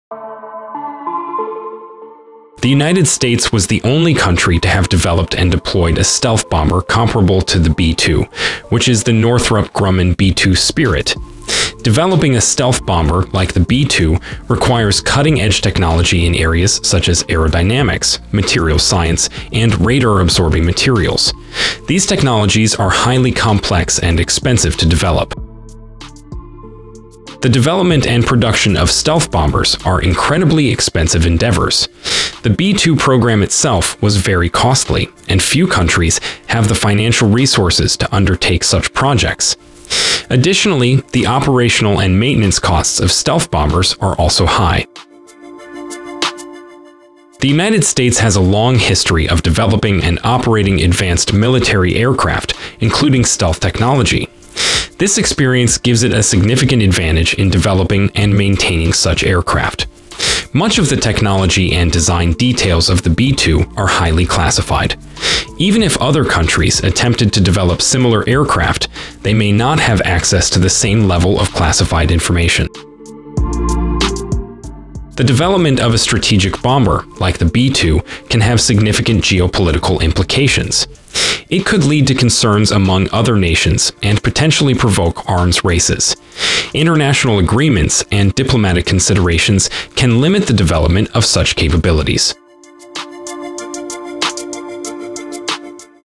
Why Has No other Country Developed a stealth Bomber Comparable to the B-2?
the United States was the only country to have developed and deployed a stealth bomber comparable to the B-2, which is the Northrop Grumman B-2 Spirit. Developing a stealth bomber like the B-2 requires cutting-edge technology in areas such as aerodynamics, materials science, and radar-absorbing materials. These technologies are highly complex and expensive to develop.